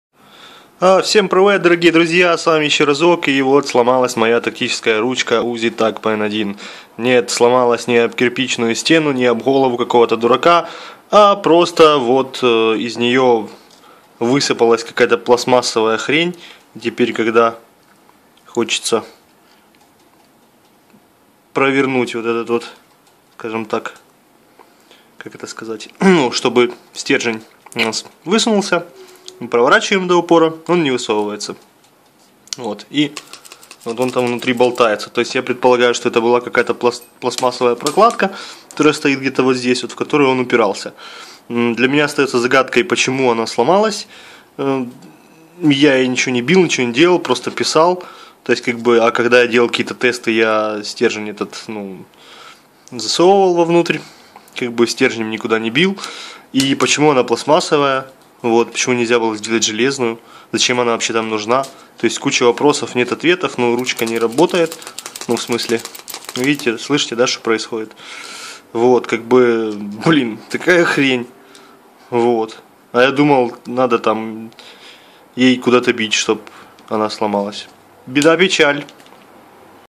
Тактическая ручка сломалась
Моя Узи Сломалась! И что самое печальное, не от удара...